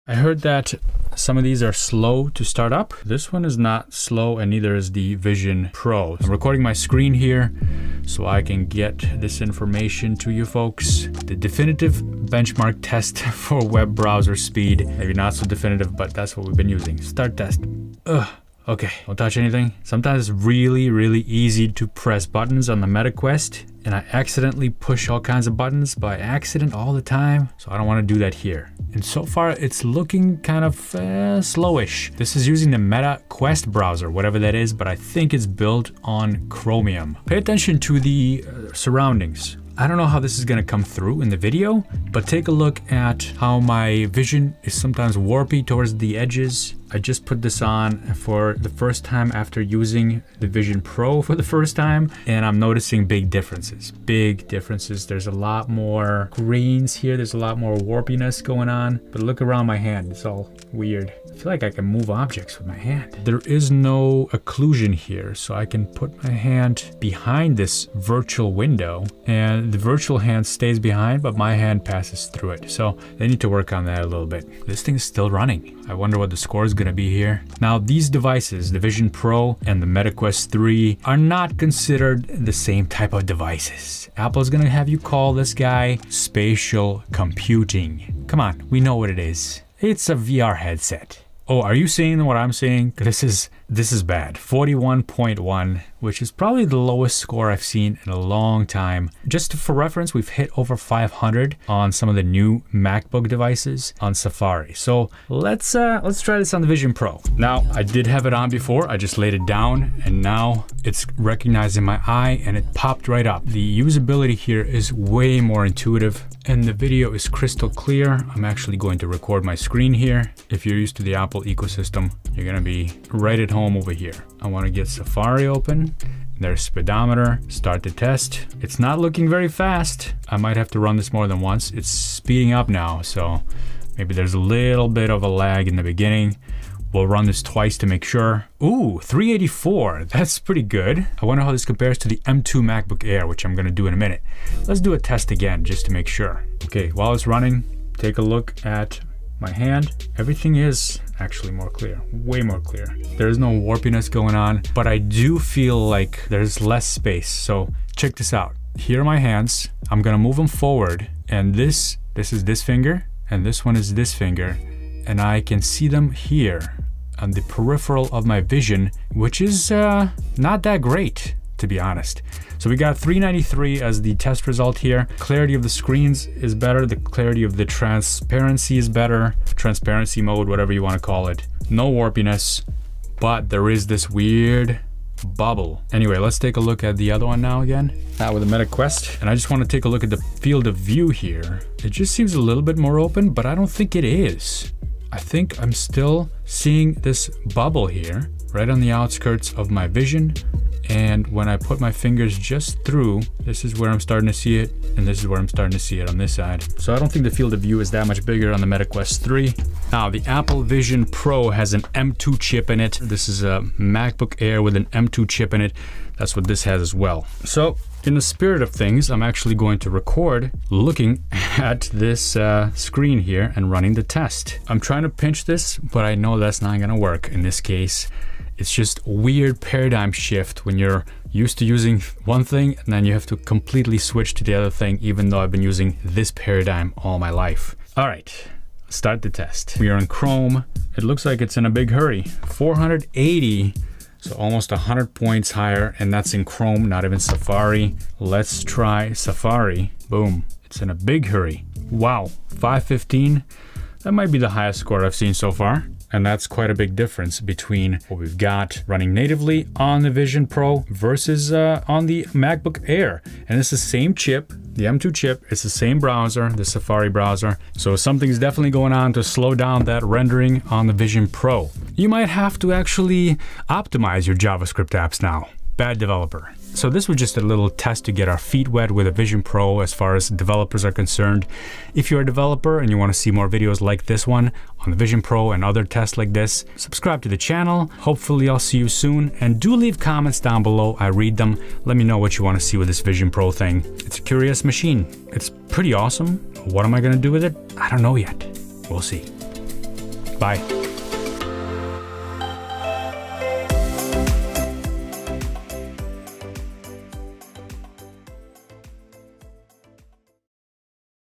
Vision Pro is pushing the M2 too hard!
I begin my Apple Vision Pro testing with the quintessential web browser speed test, and there's something odd going on.

🛒 Gear Links 🛒
* 🍏💥 New MacBook Air M1 Deal
* 💻🔄 Renewed MacBook Air M1 Deal